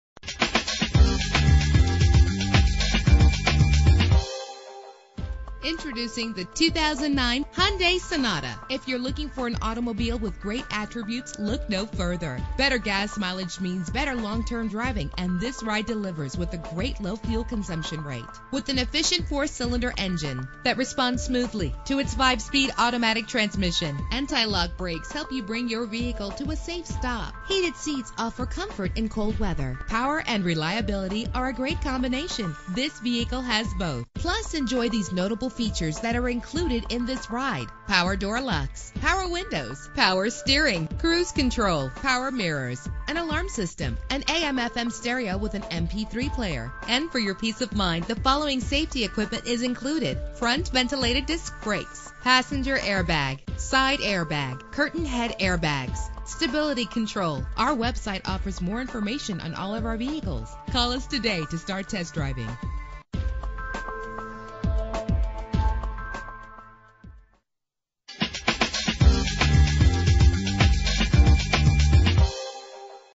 2009 Hyundai Sonata - Bountiful UT
Year: 2009
Make: Hyundai
Model: Sonata
Trim: GLS
Engine: 2.4 liter inline 4 cylinder 16 valve
Transmission: 5-Speed Automatic
Color: White
Mileage: 39943
Address:
2380 South Hwy 89
Boun